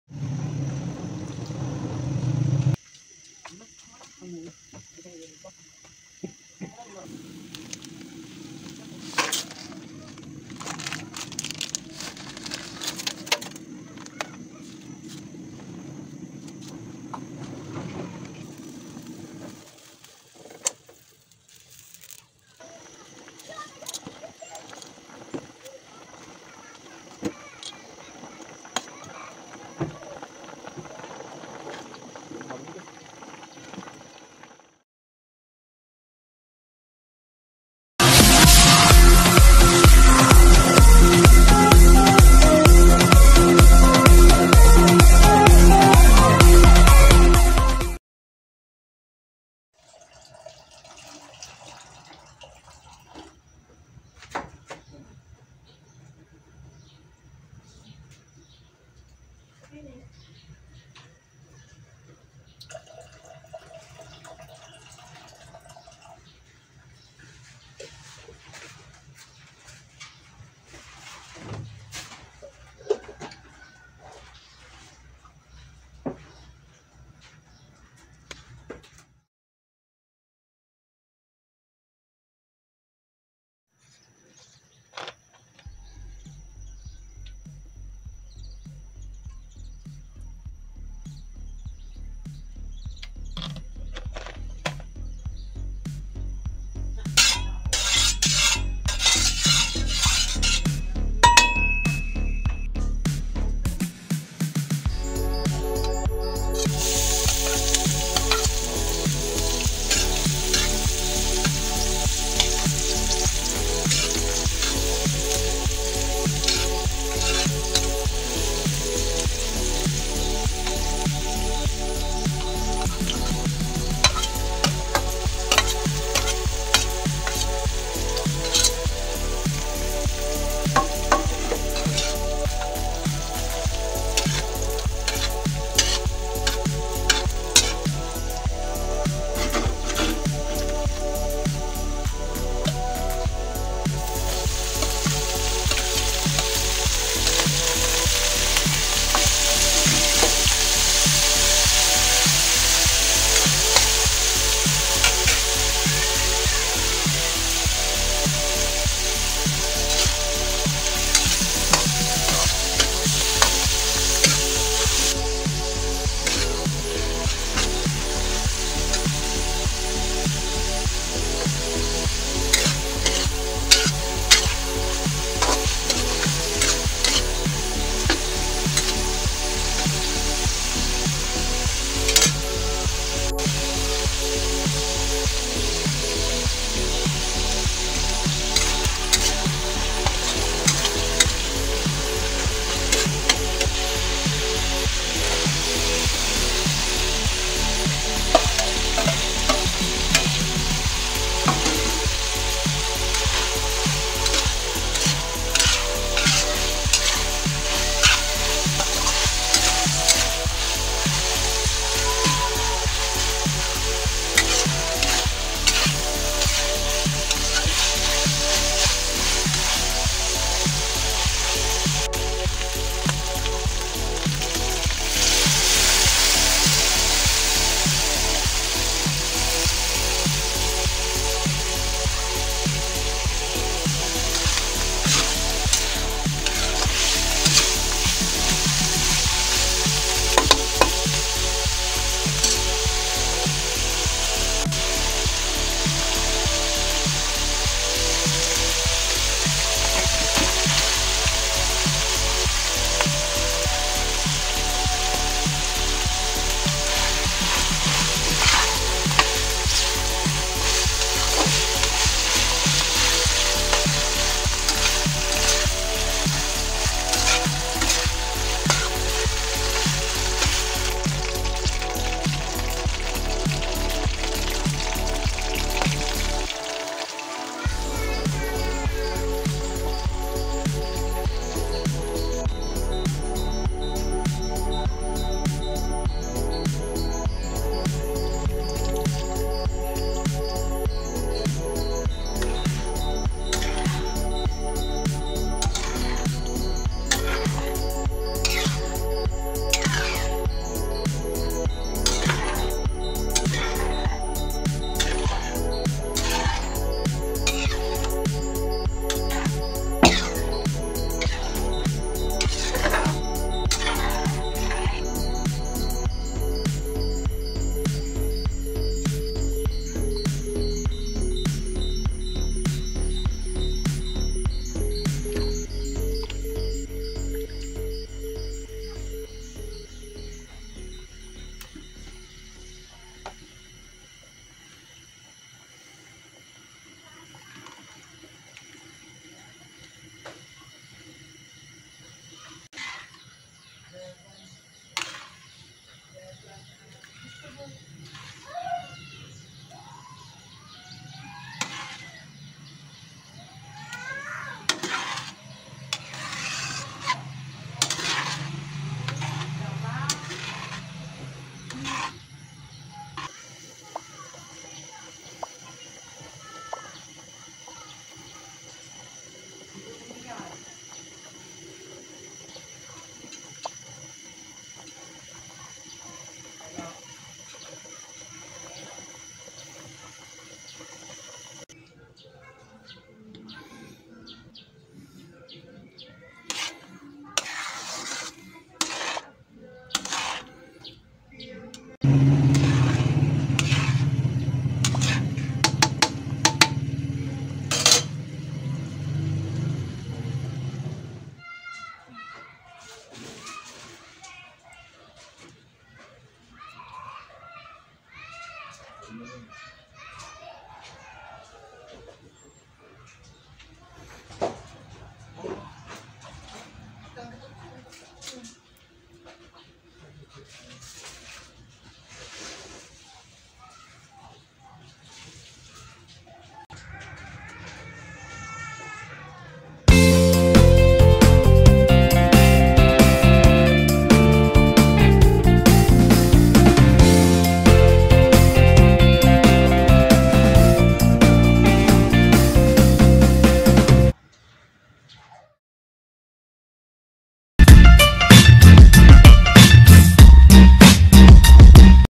CREAMY CARBONARA RECIPE
ingredients:
2 packs nestle cream
1 1/2 can evaporated milk
2 tlbs. of white sugar
minced garlic
minced onions
ground black pepper
1/4 ground pork
cheese ( 1/2 cup grated)
1 370ml. can of sliced mushroom
1 can cream of mushroom
oil and salt to taste
800 grams. of pasta
5 sliced of ham